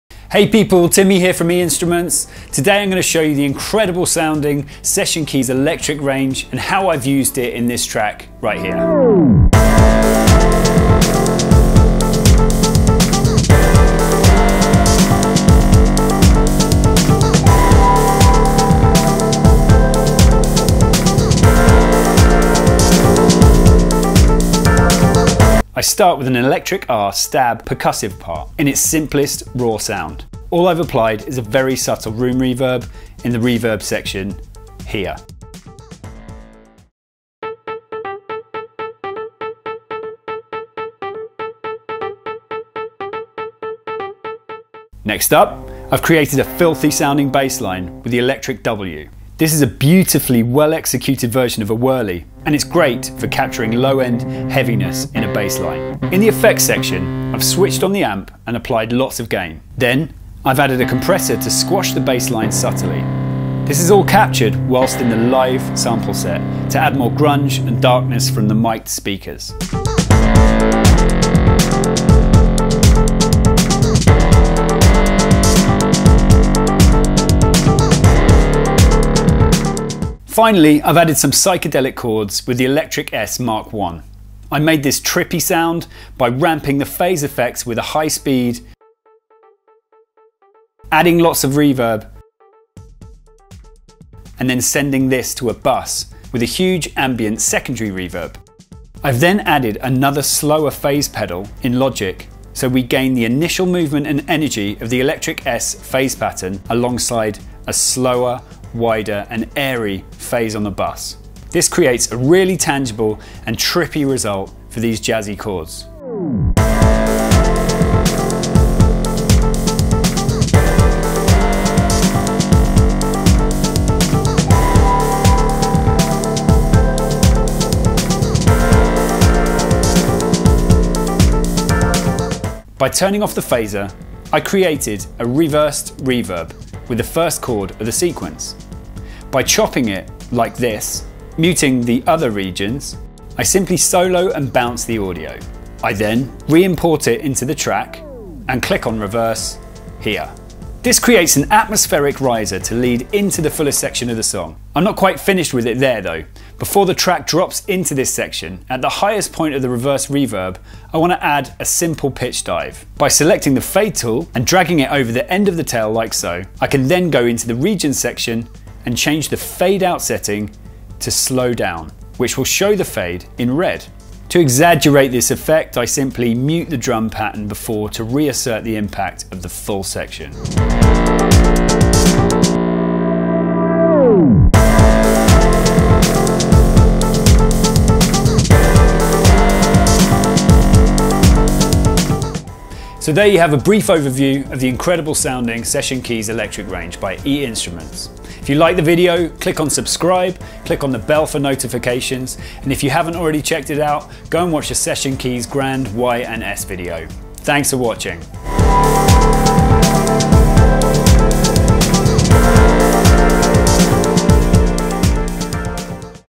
Session Keys Electric Range - Production Tips
Session Keys Electric R, Electric W, and Electric S

Timmy at e-instruments reveals how he has included all three of the Session Keys Electric instruments as separate elements within this track, including a rhythmic staccato pattern, a gritty, distorted bassline, and a phased chord pattern.

Not only does he show how each part is created within the instrument but also thrown in some neat production tips as an added bonus for you to try out in your own productions.